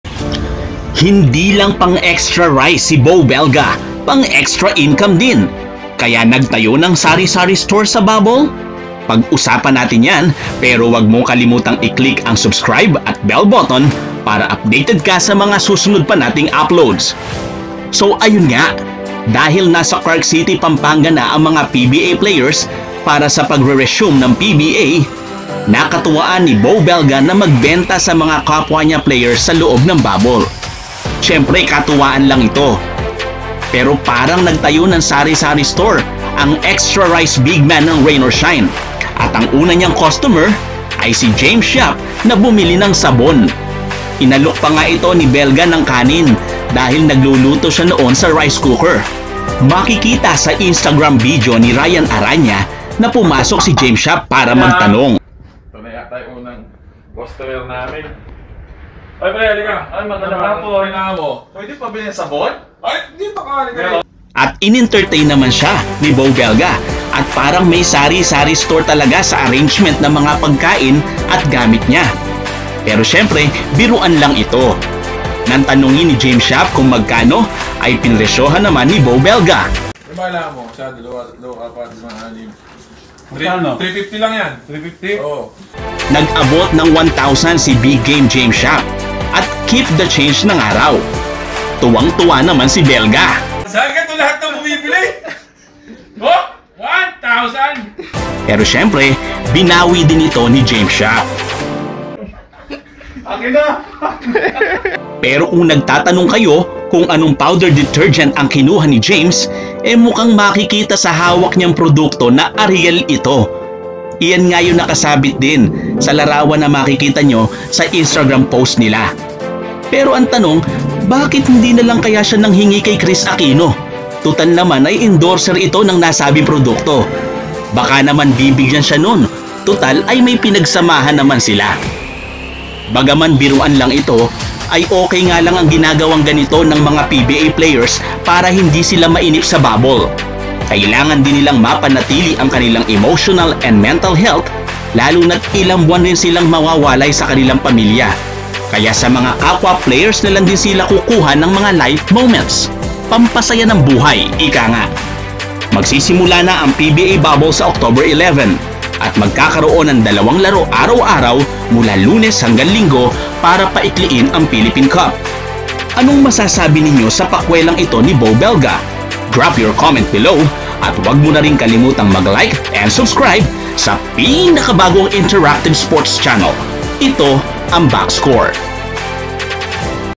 BEAU BELGA and JAMES YAP Nagnegosyo sa PBA Bubble| Beau Belga nagtinda,James Yap bumili!
BEAU BELGA and JAMES YAP Nagnegosyo sa PBA Bubble| Beau Belga nagtinda and  James Yap bumili. Alamin ang produkto ni Beau Belga na binili ni James yap

Bakit kaya naisipan ni Beau Belga na magnegosyo?